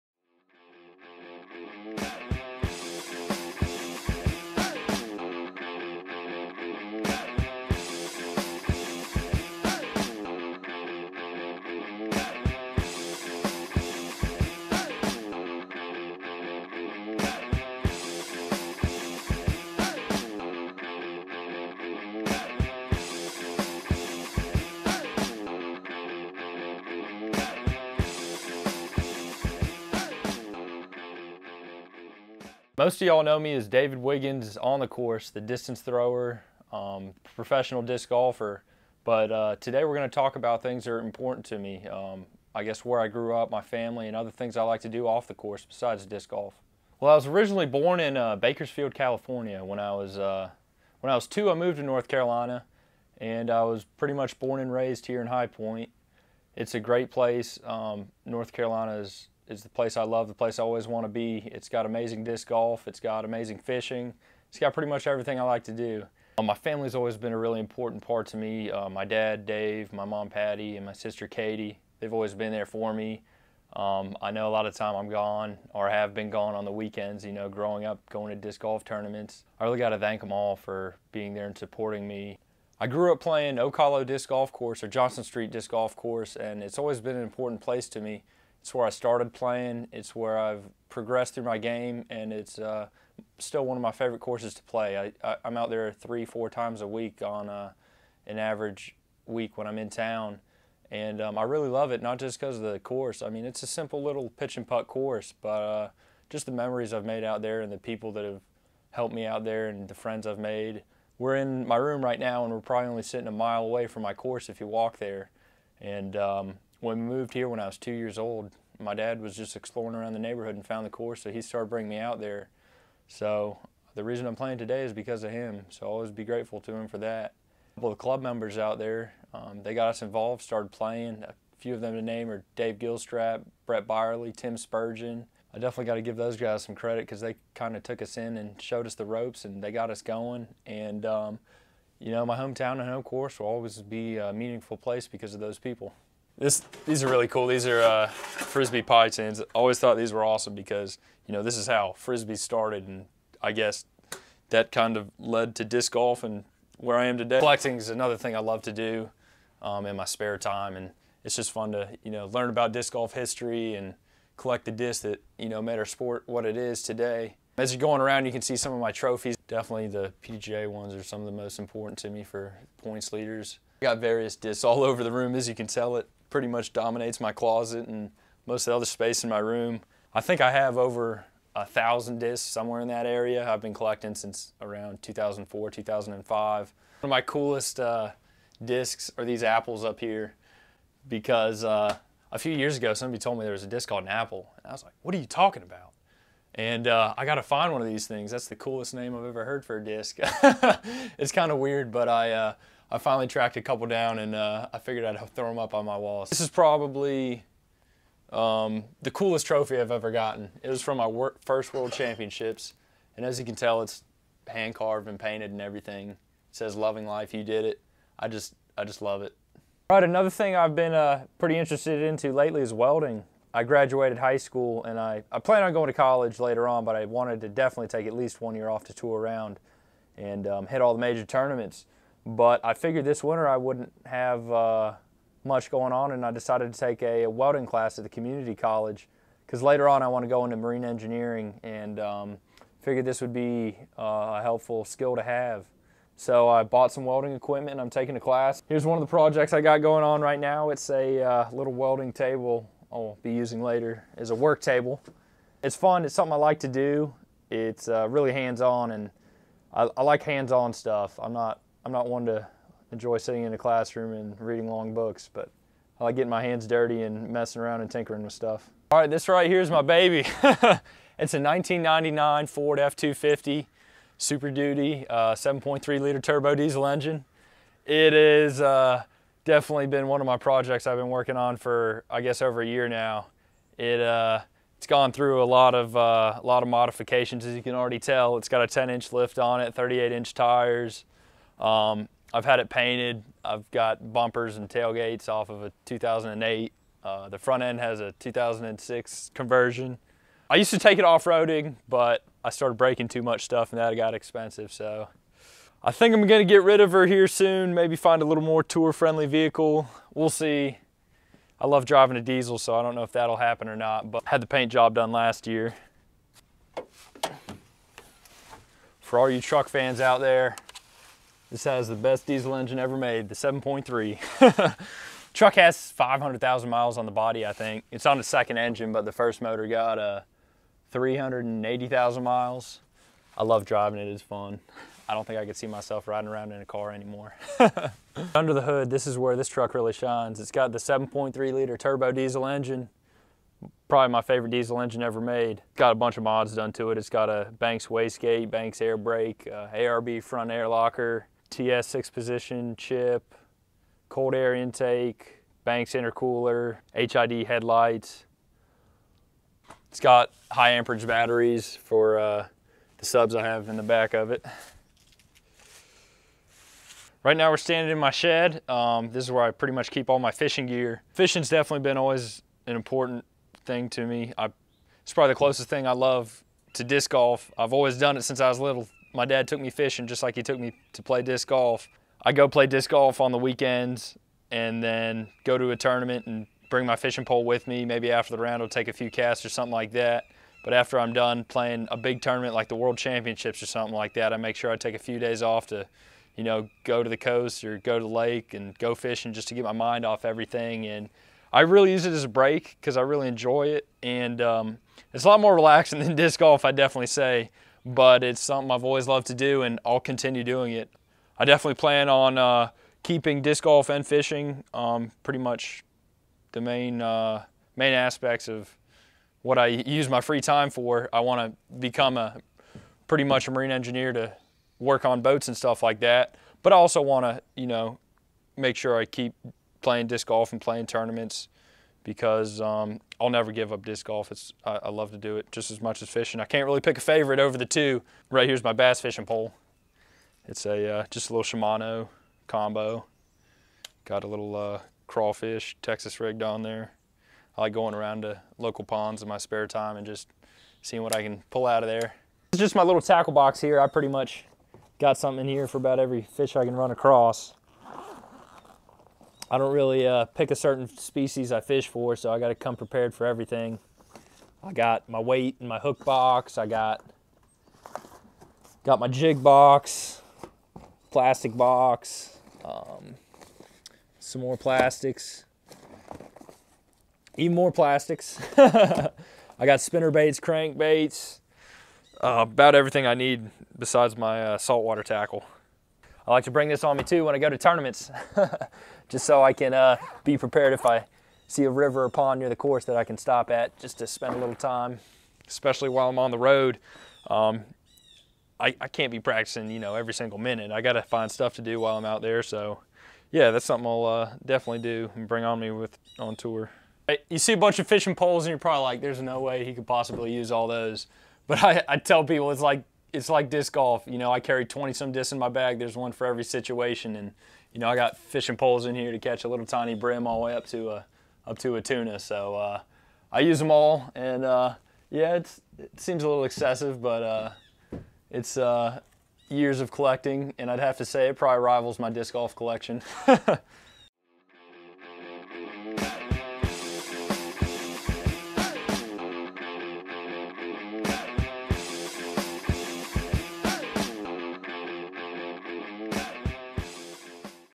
Going the Distance with David Wiggins, Jr.: Episode 4: What Matters to Him
In this episode of "Going the Distance," David Wiggins discusses the interests and hobbies that occupy his time off the disc golf course.  The world distance champion talks about disc collecting, welding, his truck, and fishing--the things that make him a complete person, not just a world champion golfer.